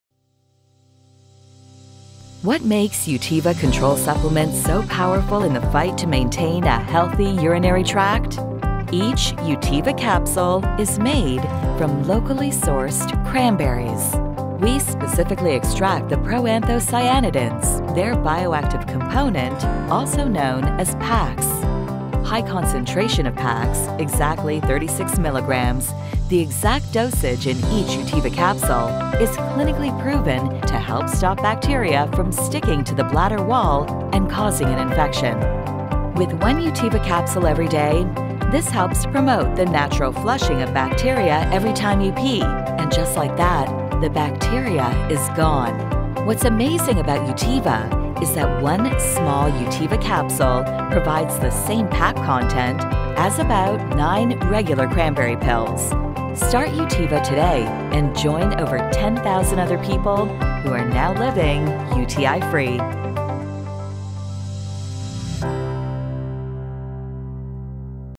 Utiva - Cranberry PAC Explanatory Video. With Subs
Learn how cranberry PACs work to naturally prevent urinary tract infections (UTIs). Utiva provides 36mg of cranberry PACs (active molecules) in each capsule measured scientifically by DMAC/A2 to ensure the best quality for urinary tract health.